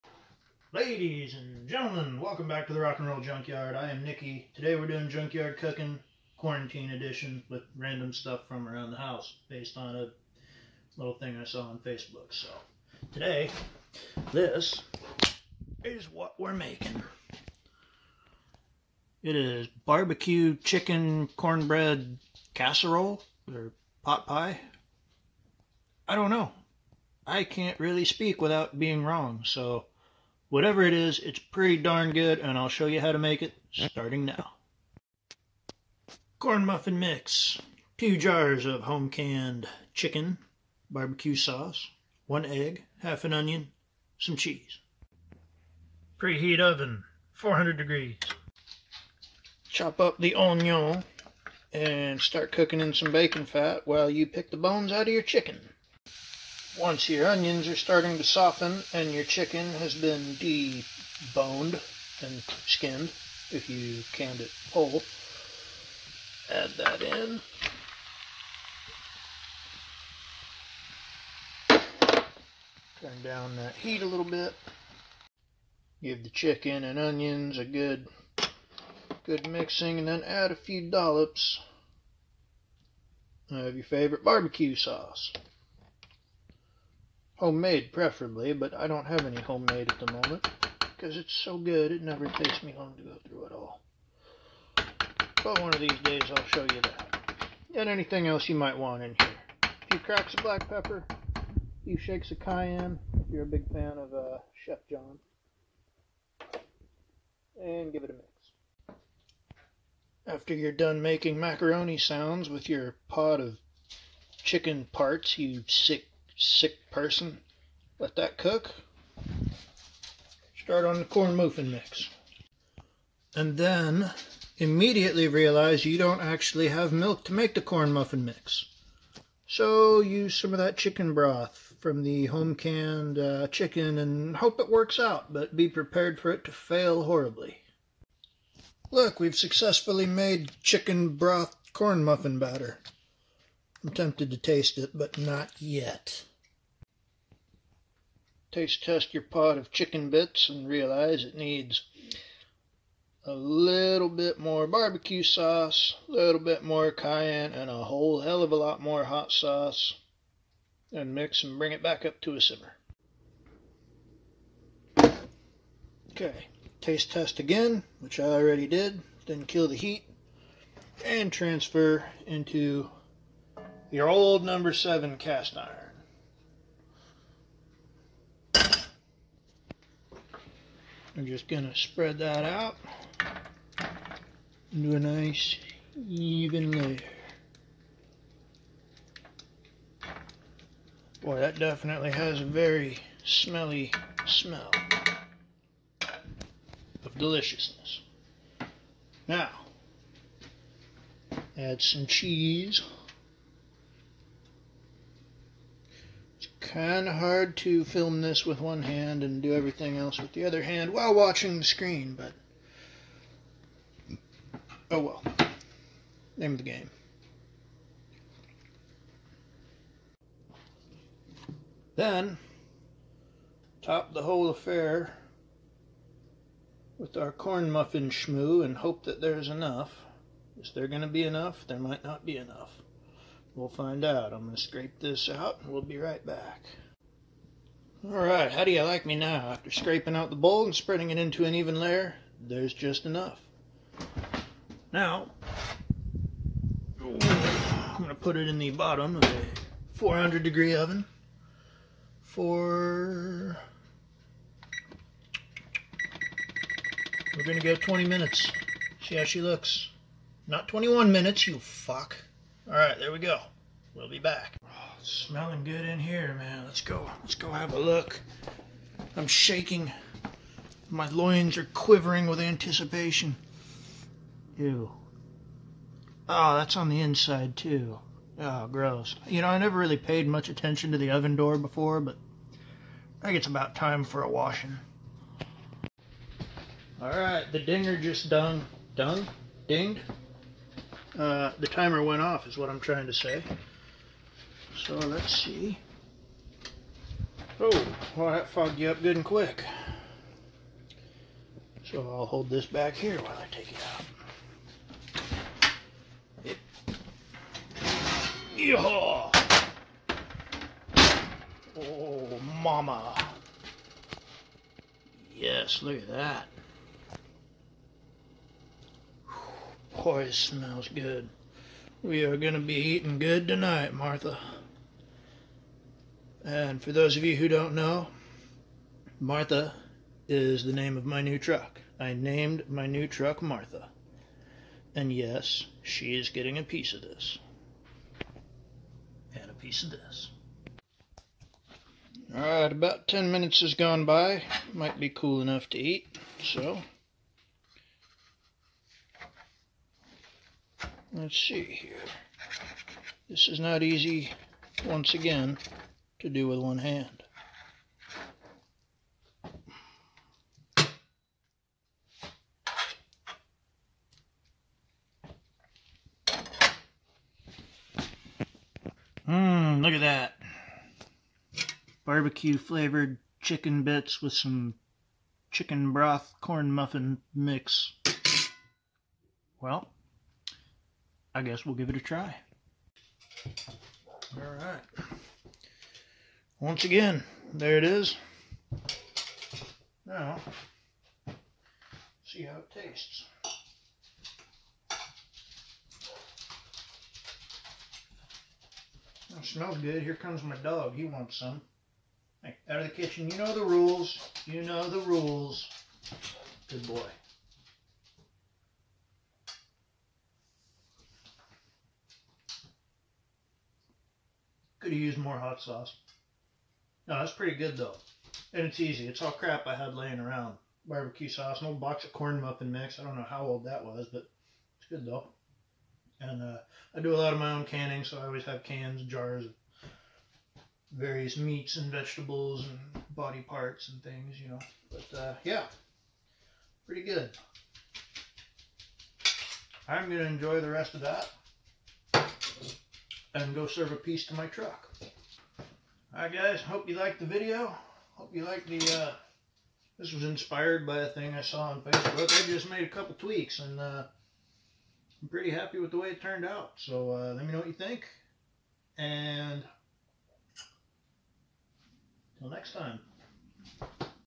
Junkyard Cookin' - Quarantine Edition - Delicious BBQ Chicken Cornbread... thing.
BBQ chicken cornbread casserole, maybe? More of a pot pie? Some kinda chicken meat cake? I don't know, but whatever it is, it's darn good.